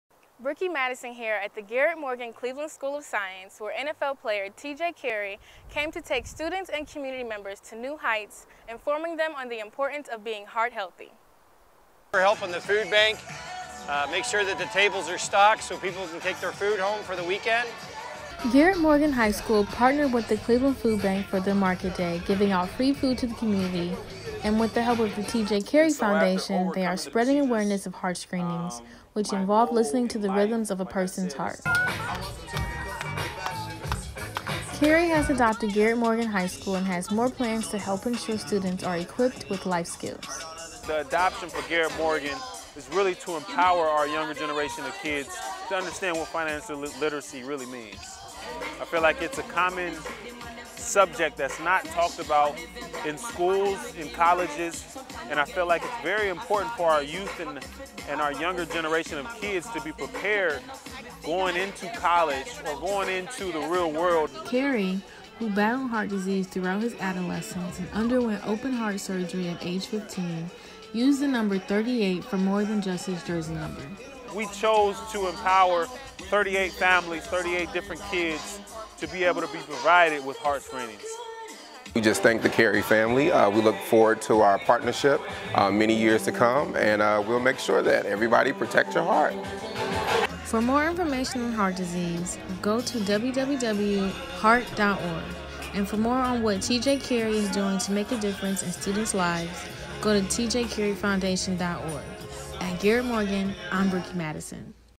Cleveland Brown's TJ Carrie adopts local high school with foundation and aids in heart screenings
As part of the TJ Carrie Foundation, Cleveland Browns player, TJ Carrie, adopted Garrett Morgan High School and hosted a heart screening event in September 2019.